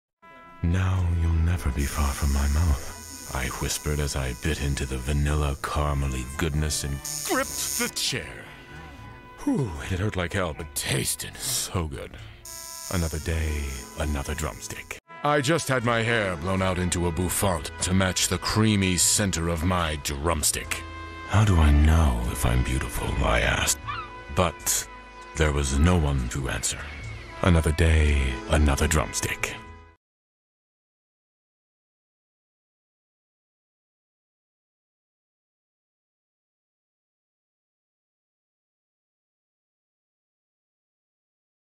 Drumstick Commercial 2022 Punky's Tattoo Parlor
Drumstick Commercial 2022 Punky's Tattoo Parlor. You can watch Drumstick commercial. There is an animated male character with a mustache, red biker shades and plaid pants takes his life-sized Nestle Drumstick to the hair salon to achieve a bouffant to match the alleged creamy center of his cone. It is a funny commercial of Drumstick..

Drumstick commercial
Drumstick